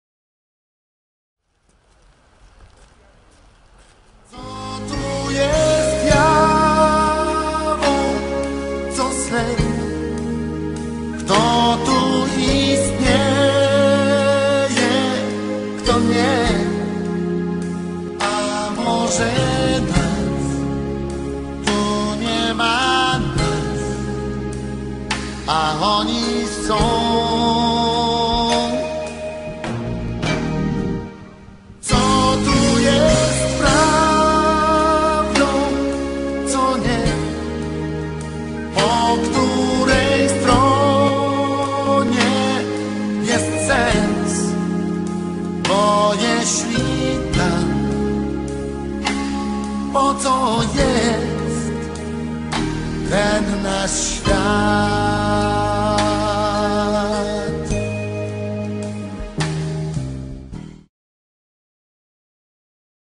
Dzień Wszystkich Świętych 2016 w Pelplinie - TV Pelplin HD
Pamięci Wszystkim Mieszkańcom Pelplina i jego okolic, którzy odeszli do Wieczności.